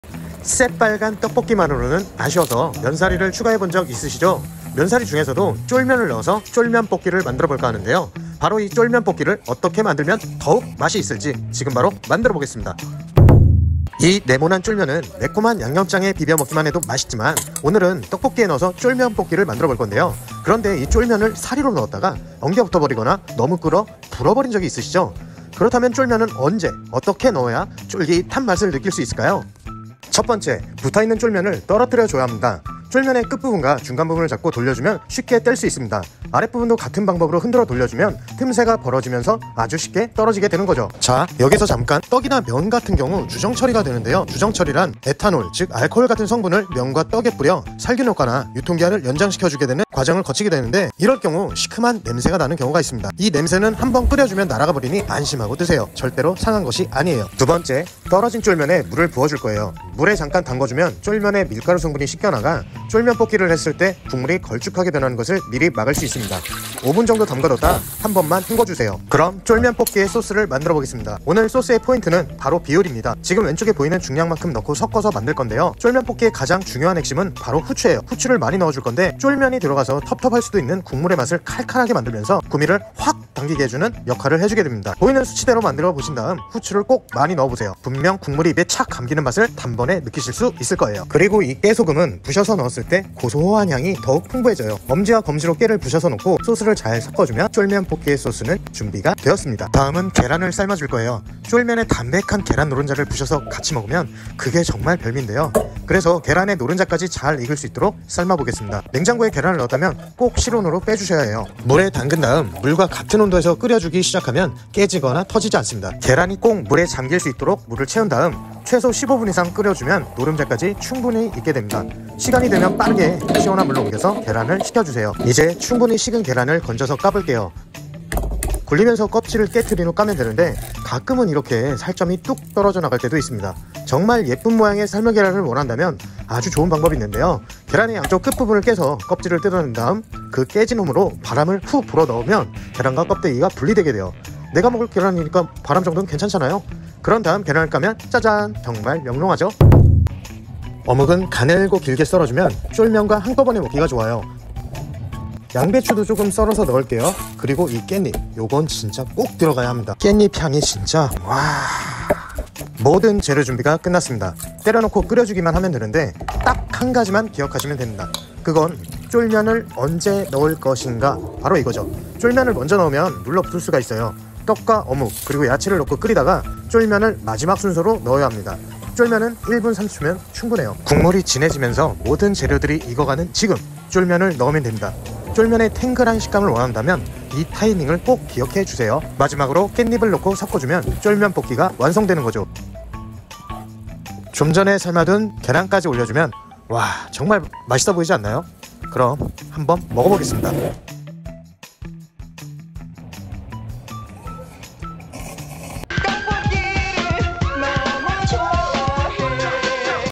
1분 30초 , 쫄볶이의 법칙 |Korean food tteokbokki
쫄면으로 쫄볶이를 만들어봤습니다
쫄면을 가장 쫄깃하게 먹을 수 있는 방법을 영상으로 찍어봤습니다